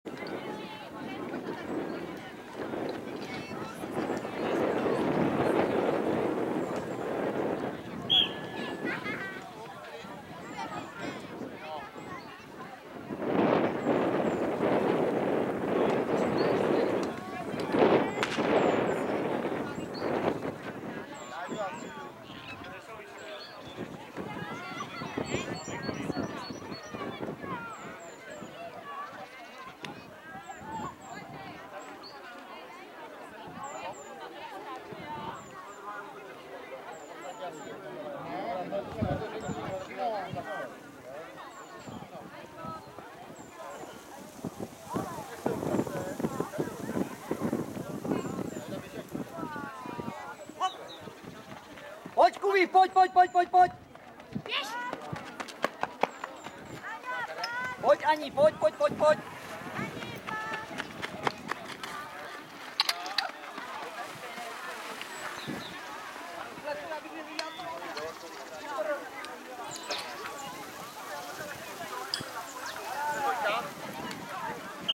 SDH Výčapy MH - mladší - štafeta 4x60m na okresním kole v Petrovicích
Štafeta A - 54,35 (2.místo)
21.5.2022
Okresní kolo MH